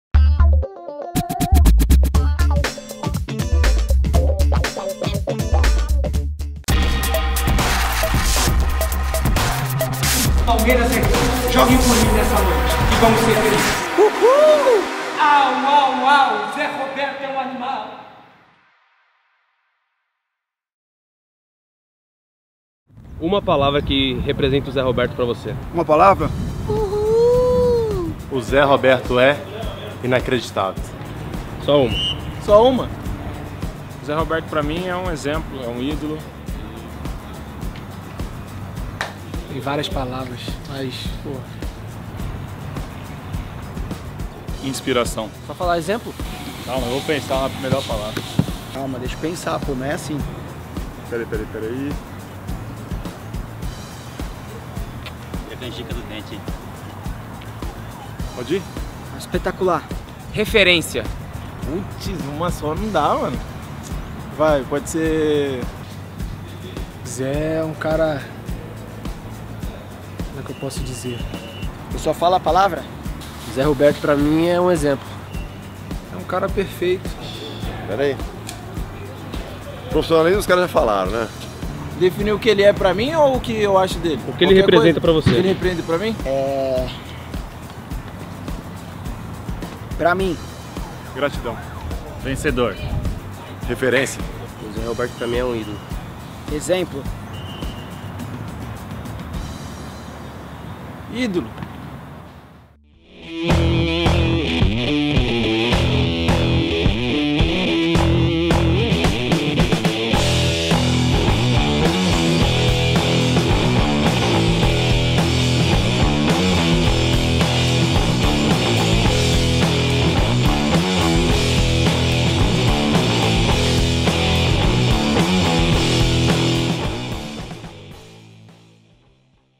Desafio: Elenco define Zé Roberto em uma palavra
SemanaZéRoberto: A TV Palmeiras/FAM começou nesta segunda-feira uma semana de homenagens a Zé Roberto.

O vídeo desta terça (5/12) mostra o elenco do Verdão definindo o ídolo Zé Roberto em uma só palavra.

Será que eles conseguiram?